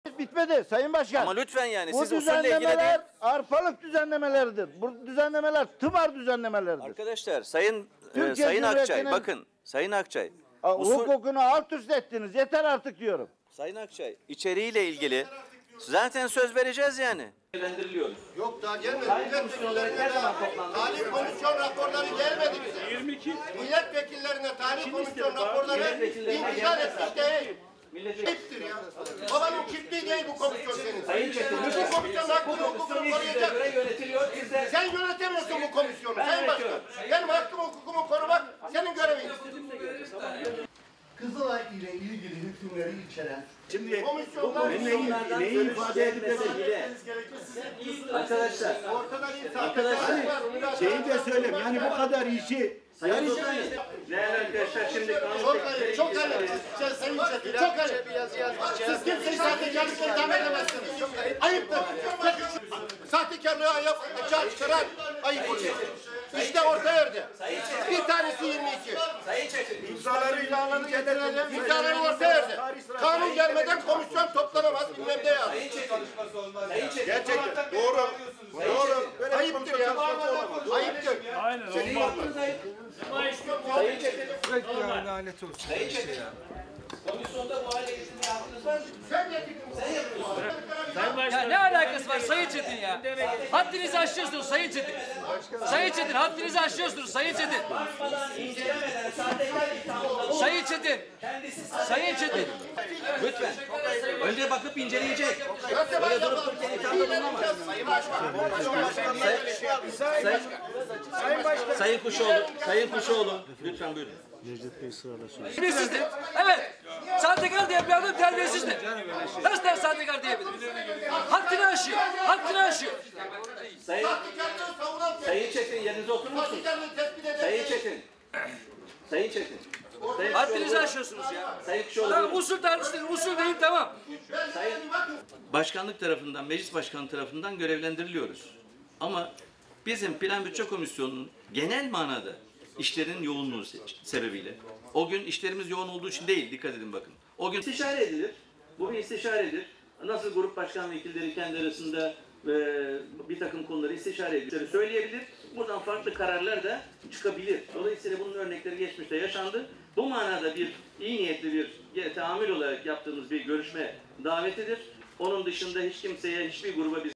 Plan ve Bütçe Komisyonu'nda 'sahtekarlık var' tartışması
Türkiye Büyük Millet Meclisi (TBMM) Plan ve Bütçe Komisyonu'nda, yeni torba yasa görüşmelerinde muhalefet milletvekilleri, kanun tekliflerinin birçok kurumu ilgilendirdiğini ve başka komisyonlarda da görüşülmesi gerektiğini veya alt komisyondan geçmesini talep etti. Bu teklifi iktidar milletvekilleri kabul etmedi. Bunun üzerine sözlü tartışmalar başladı. CHP Ankara Milletvekili İzzet Çetin "Ortada bir sahtekarlık var." diye bağırınca komisyon bir anda gerildi.